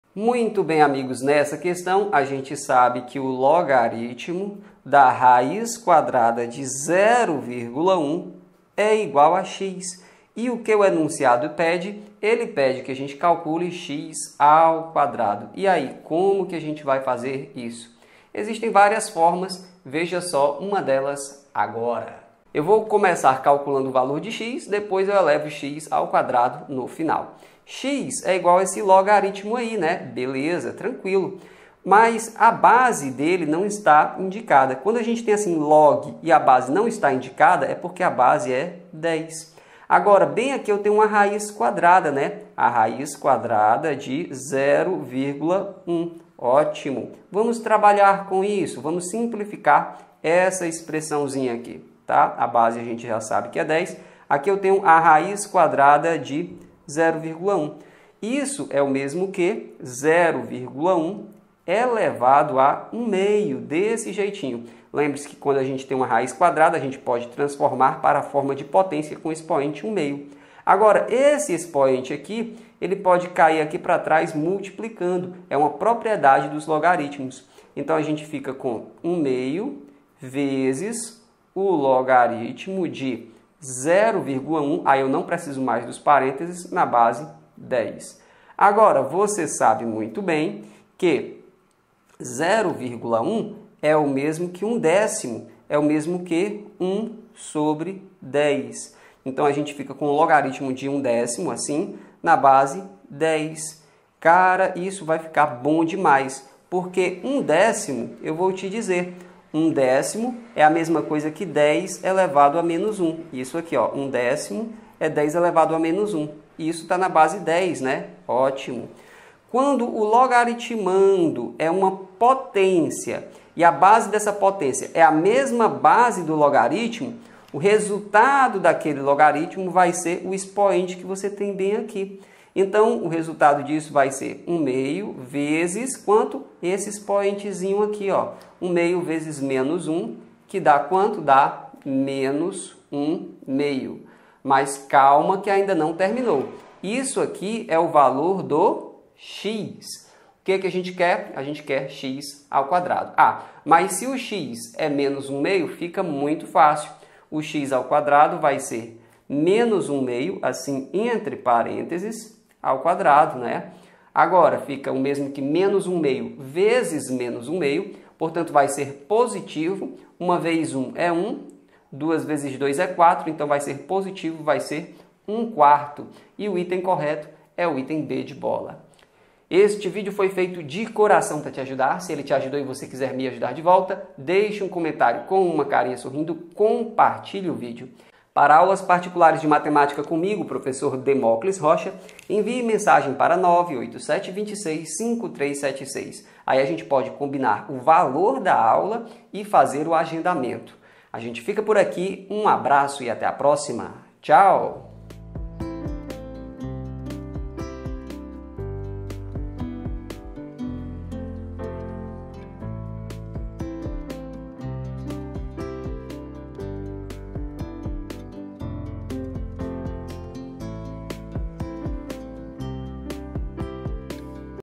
MACKENZIE - LOGARITMOS
Olá, amigos!
Professor Demóclis Rocha aqui. @matematicacomdemoclis
Neste vídeo apresento uma resolução
para uma questão de Matemática sobre logaritmos.

(MACKENZIE)
Se log (raiz quadrada de 0,1) = x,
então x² é:

Aulas Particulares de Matemática: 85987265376